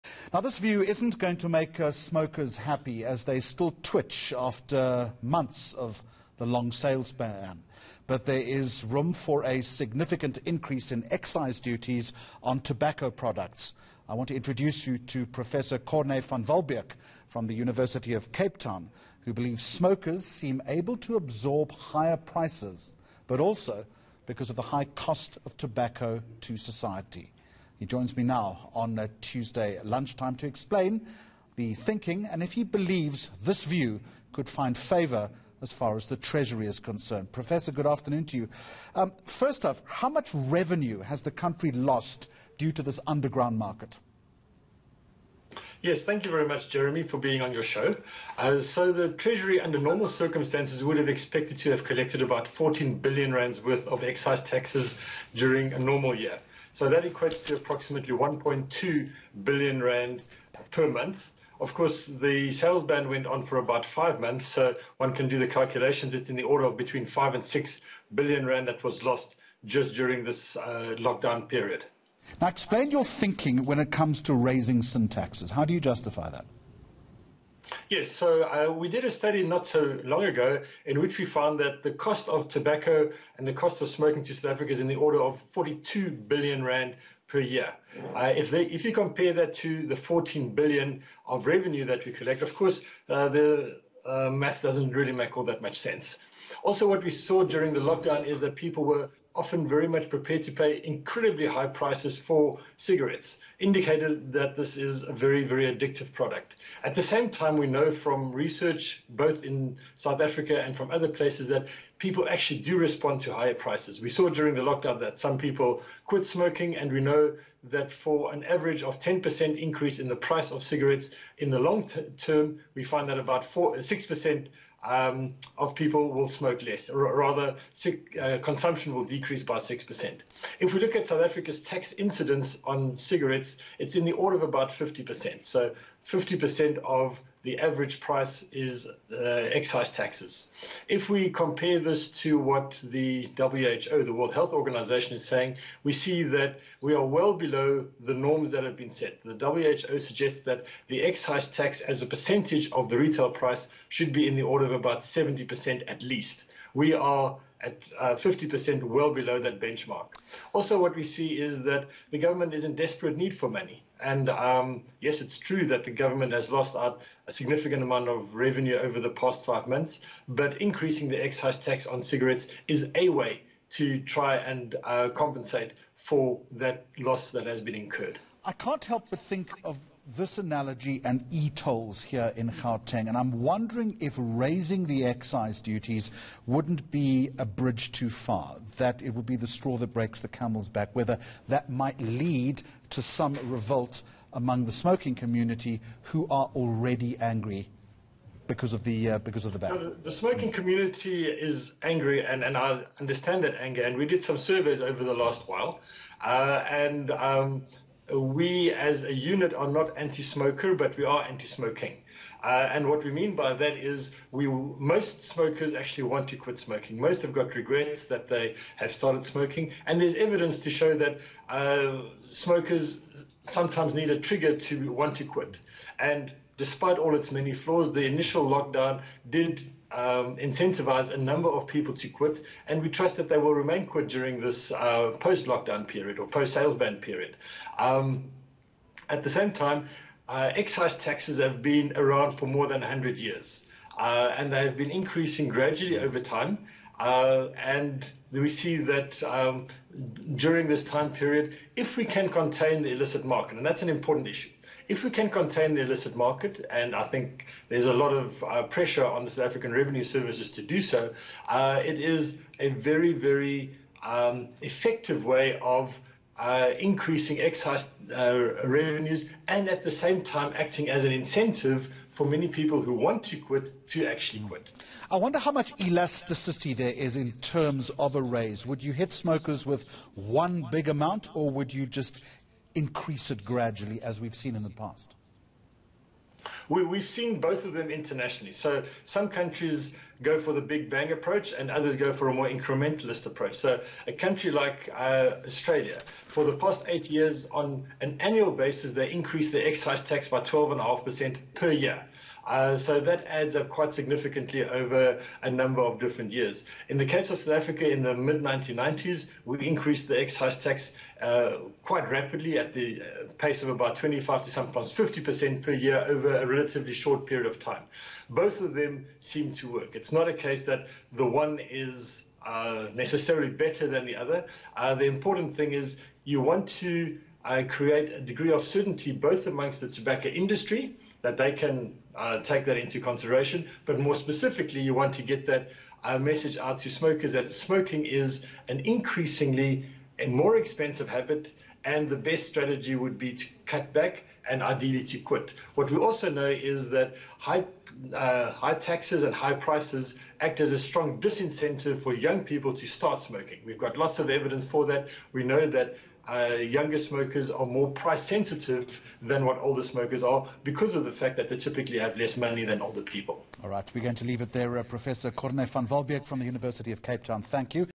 There is room to increase tobacco tax- expert
Professor Corne van Walbeek from UCT believes smokers seem able to absorb higher prices, but also because of the high cost of tobacco to society.

He spoke to Jeremy Maggs on Lunchtime to explain further. Courtesy